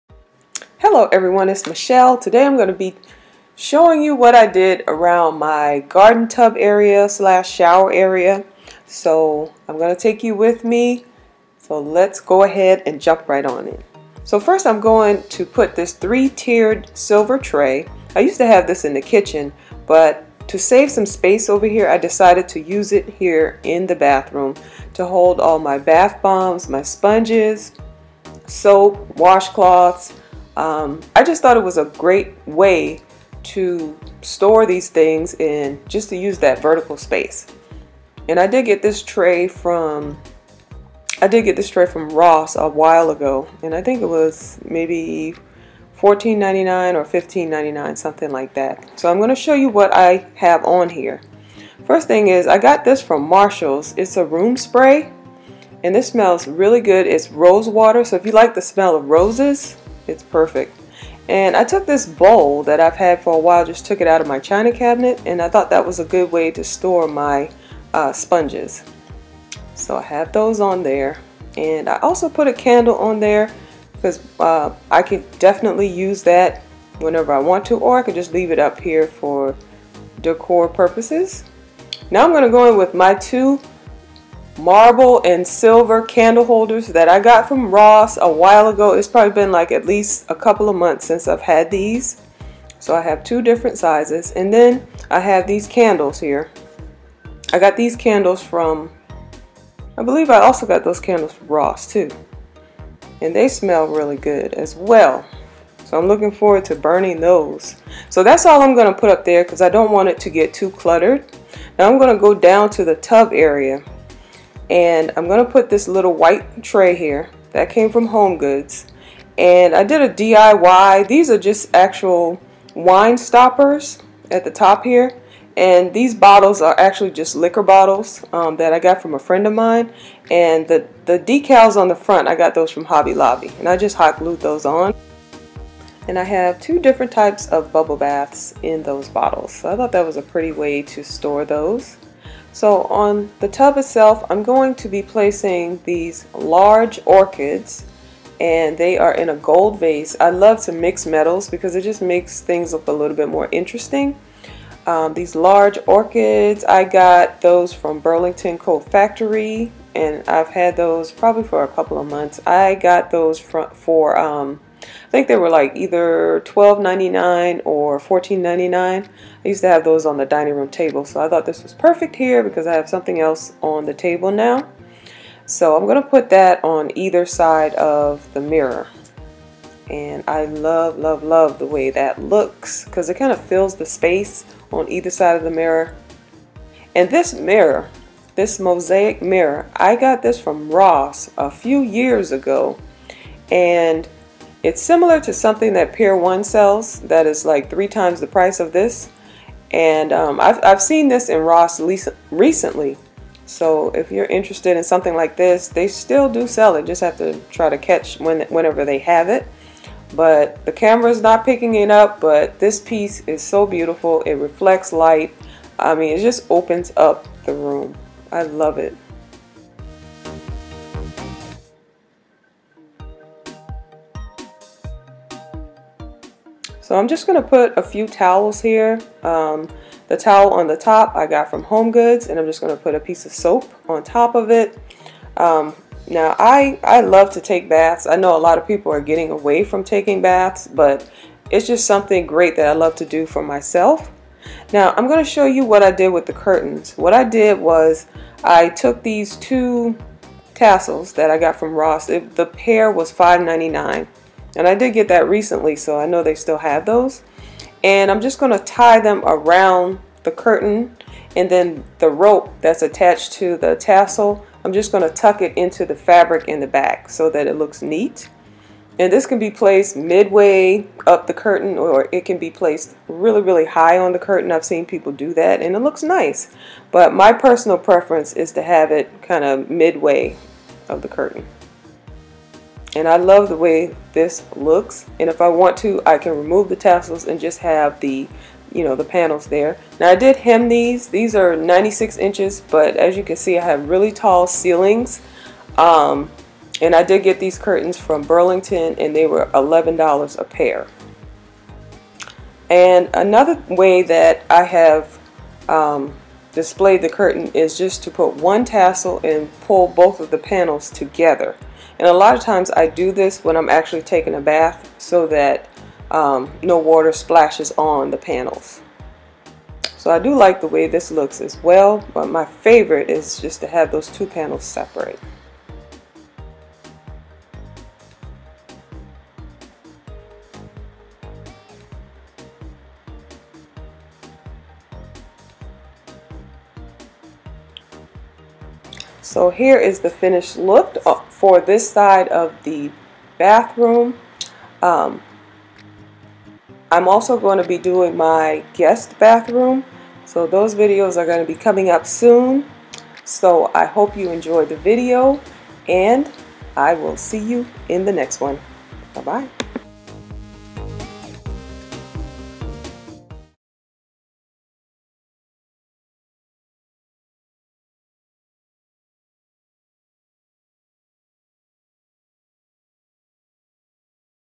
BATHROOM TUB/SHOWER AREA DECORATING IDEAS|DECORATE WITH ME
IN THIS VIDEO, I SHARE HOW I DECORATED MY BATHROOM TUB AND SHOWER AREA. I KEEP ALL OF MY BATH ESSENTIALS RIGHT NEXT TO THE TUB BECAUSE I ENJOY TAKING A BATH.

ALL ITEMS IN THE BATHROOM:
3 TIER TRAY - ROSS
TALL ORCHIDS IN GOLD VASE - BURLINGTON
WHITE TRAY - HOME GOODS
BUBBLE BATH BOTTLES DIY -
-BOTTLE - LIQUOR BOTTLES
-DECAL- HOBBY LOBBY
-WINE STOPPER - BURLINGTON
TOWELS - HOMEGOODS
WHITE CANDLES - ROSS
SILVER AND WHITE BOWL - MARSHALLS
ROSEWATER ROOM SPRAY - MARSHALLS
CURTAINS - BURLINGTON
TASSELS & CURTAIN ROD - ROSS
MOSAIC MIRROR - ROSS

THANKS FOR JOINING ME FOR THIS DECORATE WITH ME VIDEO.
MY NEXT VIDEO WILL SHOW HOW I DECORATE MY GUEST BATHROOM. SEE YA THEN!